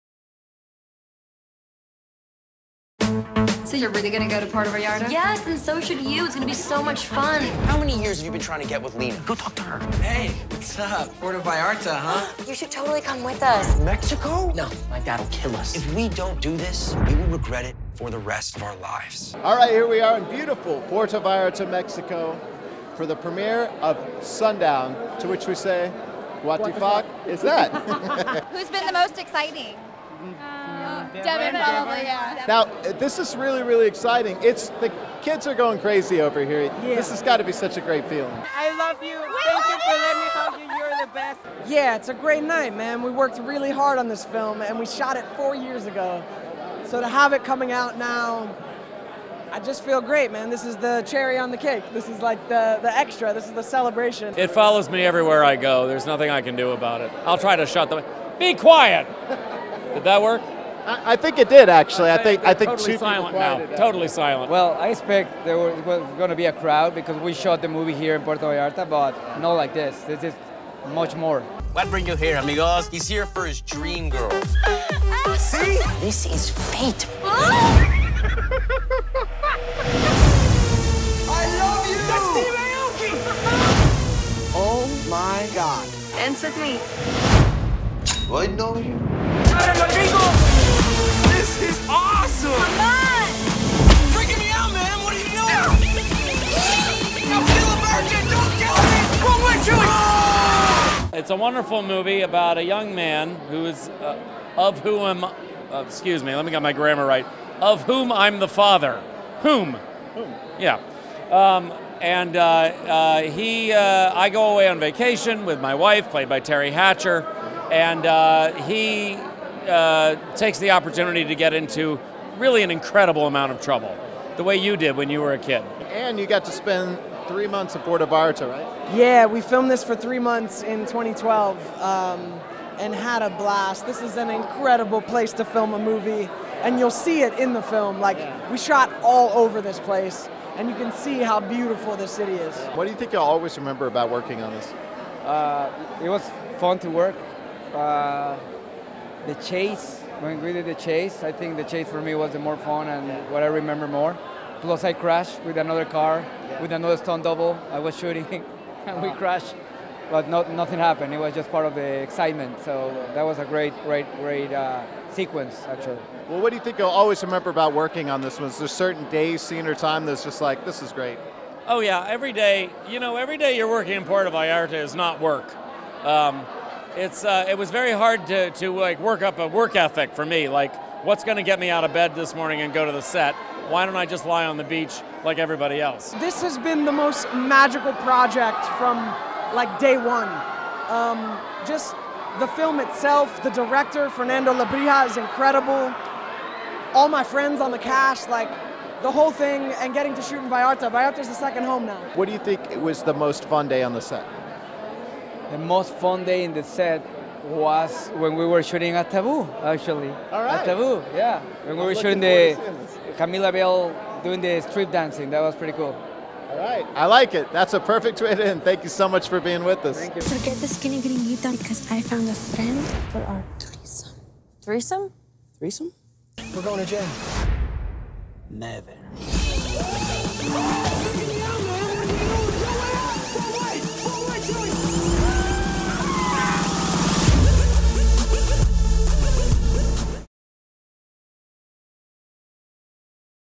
Let's Go DFW! - Sundown movie premiere in Puerto Vallarta
Paul Salfen and Ana Liza Garza interview the director and cast members from the film Sundown at the premiere in Puerto Vallarta, Mexico, where it was filmed.